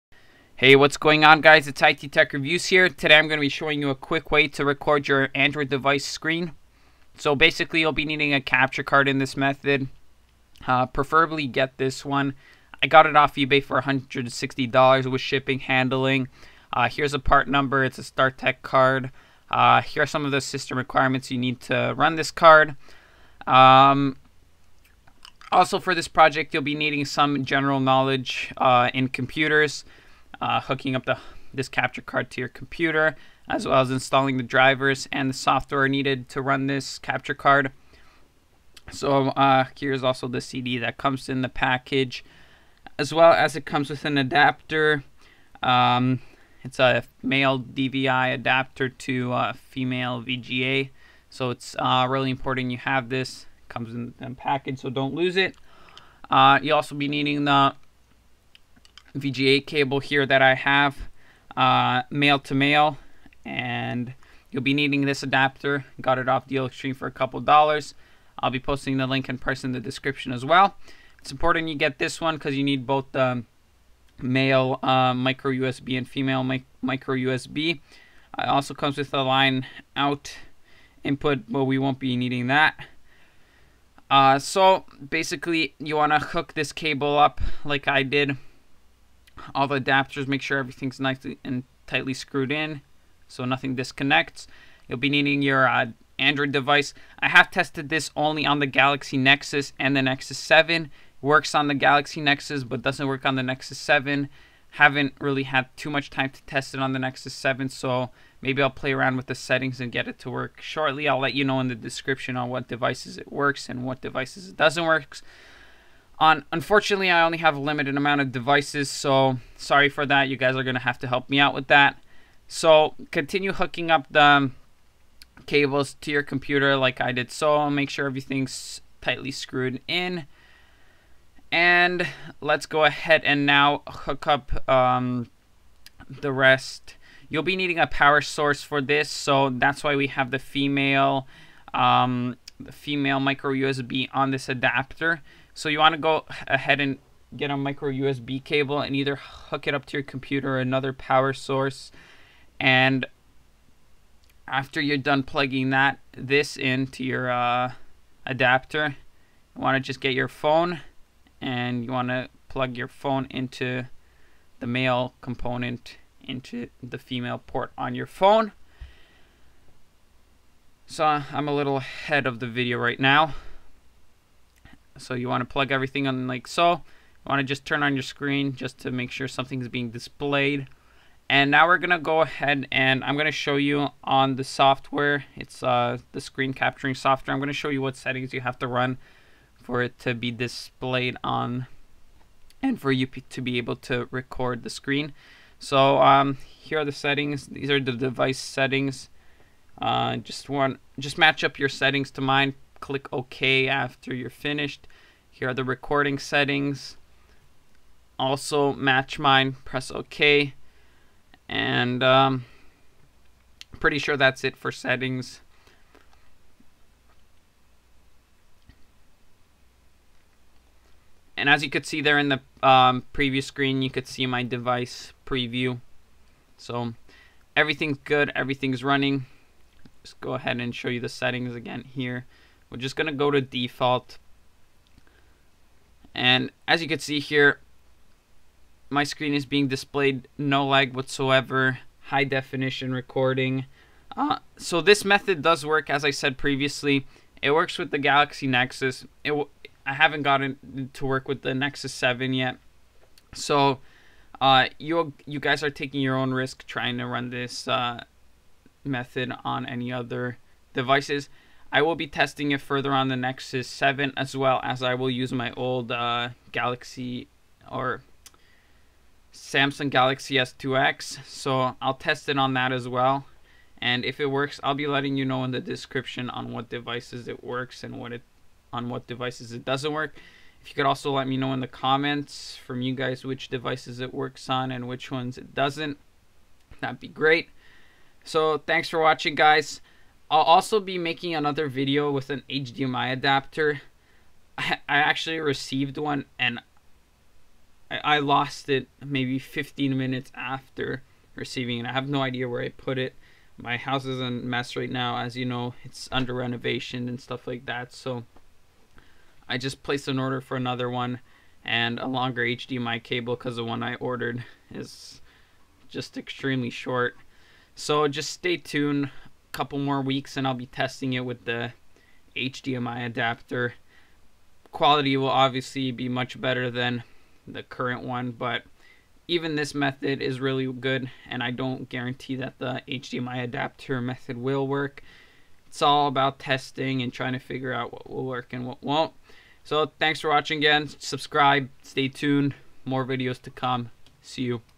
HOW TO: Record Your Android Device's Screen (NO ROOT)
Please read full discription it is very important. If you can please let me know in comments what devices it works on and which one it doesnt work on that would be awesome I cannot afford to buy anymore phones/tablets at the moment ): Please remember to like, comment, subscribe it really helps me out :)
Working on: Galaxy Nexus, Samsung Galaxy S2 X
Not Working on: Nexus 7
What you will need: